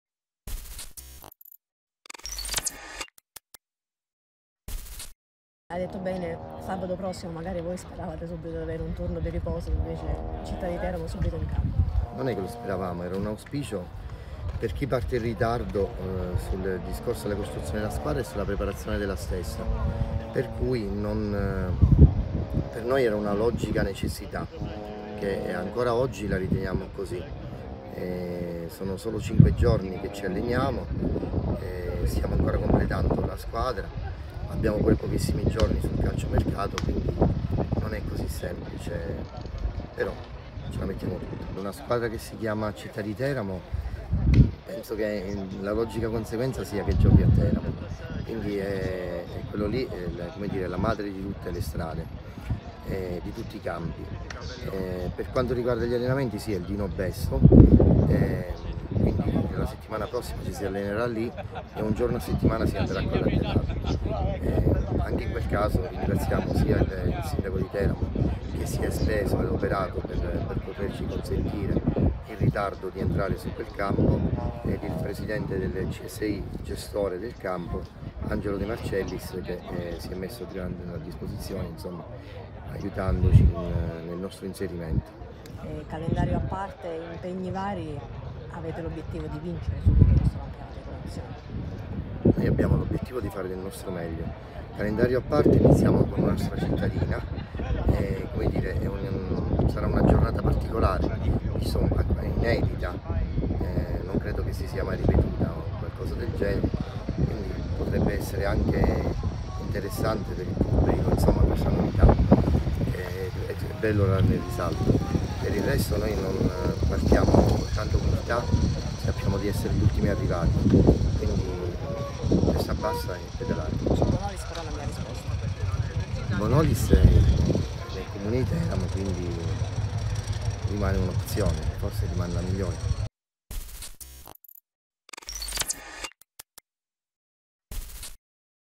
Per il Città di Teramo il Bonolis resta "l'opzione migliore" dove giocare
Il punto del presidente Eddy Rastelli a pochi giorni dall'esordio nel campionato di Promozione